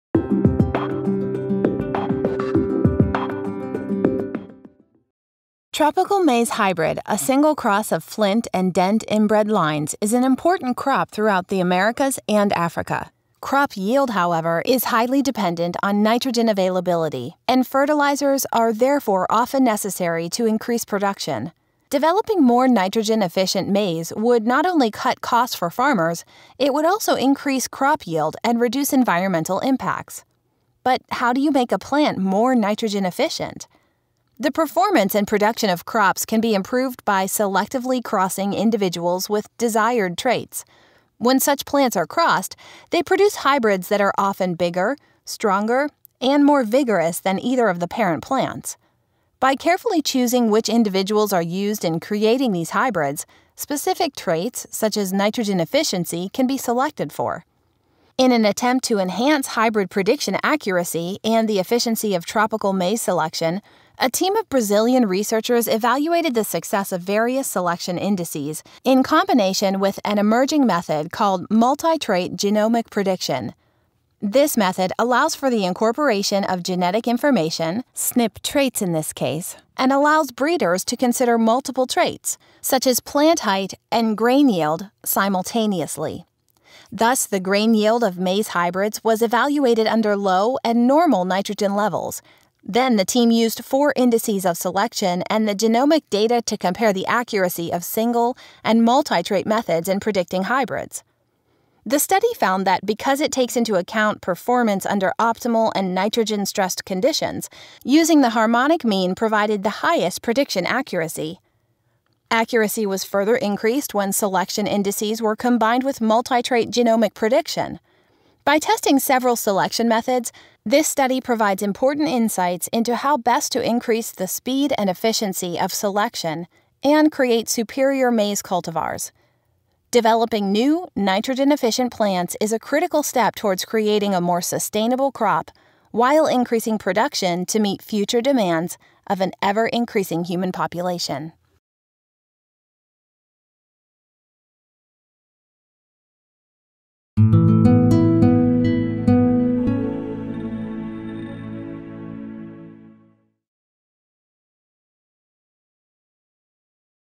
Building a better plant: Increasing nitrogen efficiency of tropical maize
Lyra et al. "Multi-trait genomic prediction for nitrogen response indices in tropical maize hybrids.” Molecular Breeding (2017). doi: 10.1007/s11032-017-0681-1